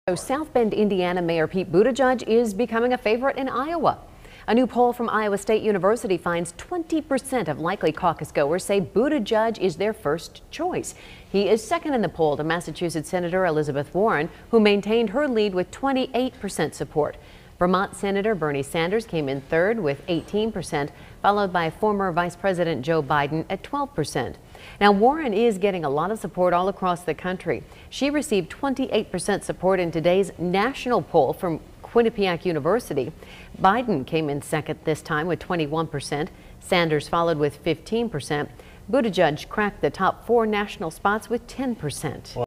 Buttigieg surges to second place in Iowa State University poll
According to the poll, 20% of voters named the Indiana mayor as their first pick.